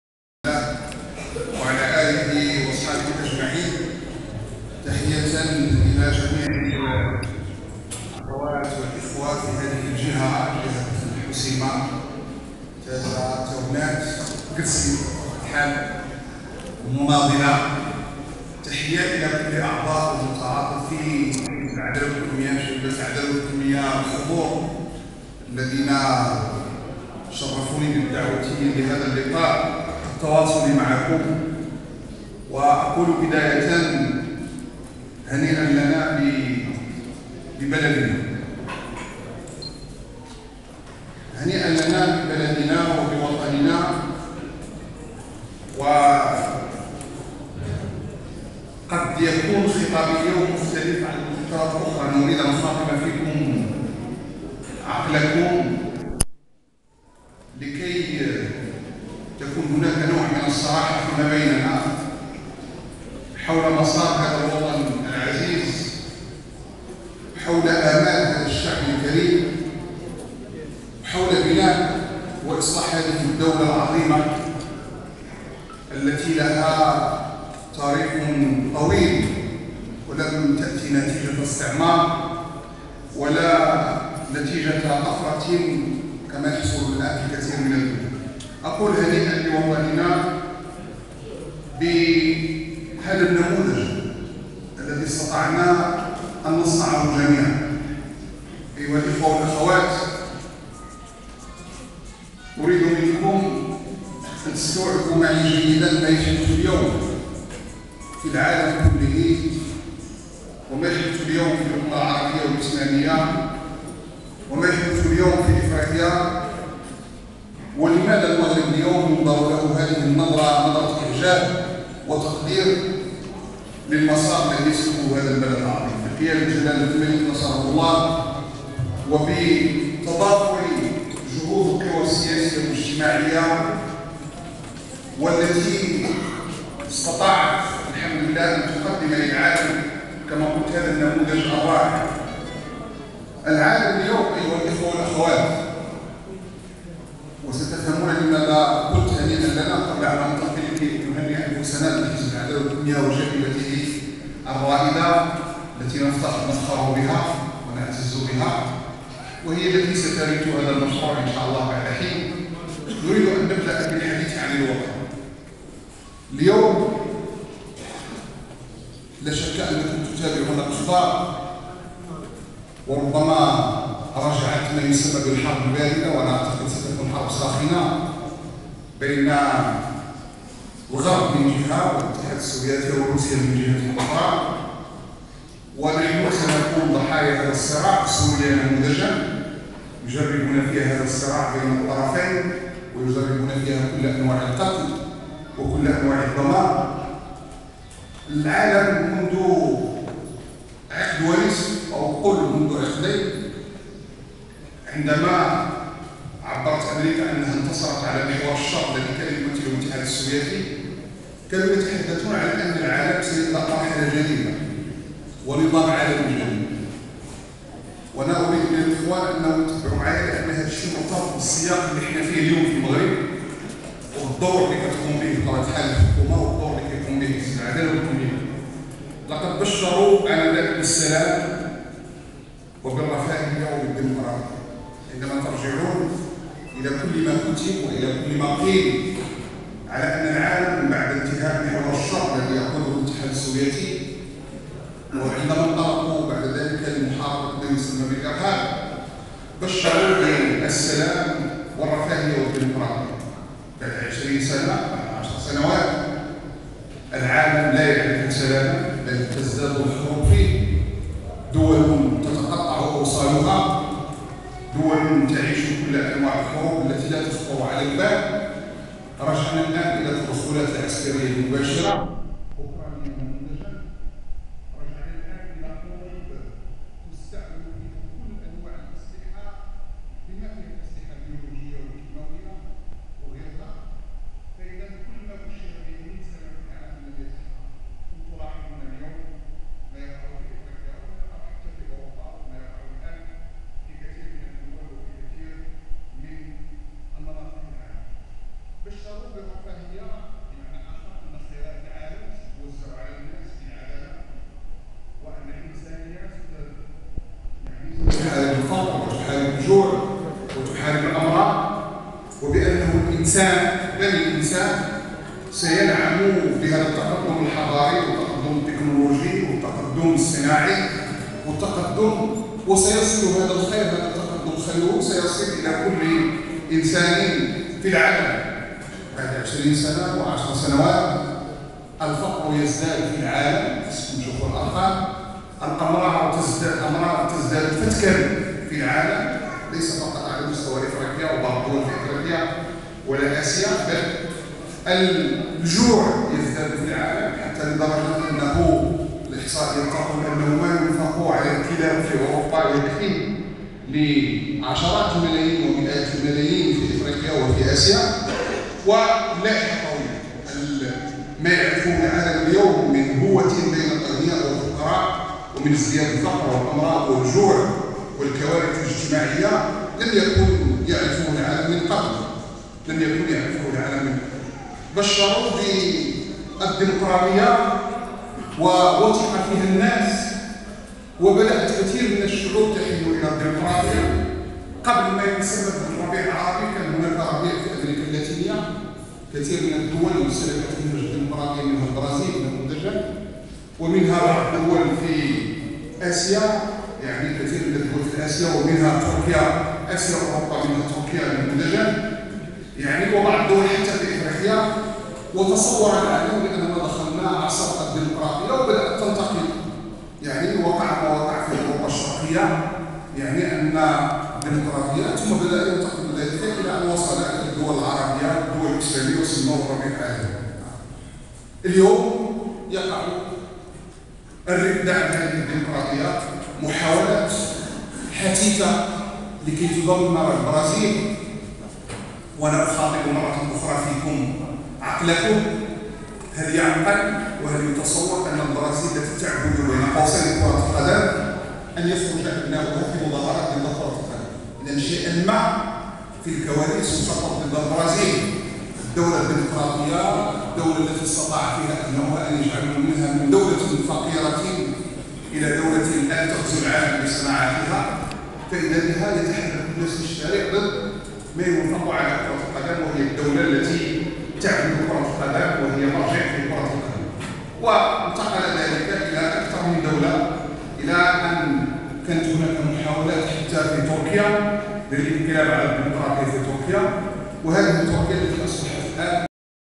عبد العزيز رباح بتازة
عبد العزيز رباح وزير التجهيز والنقل واللوجيستيك يحاضر بتازة في اطار فعاليات الملتقى الجهوي الأول لشبيبة العدالة والتنمية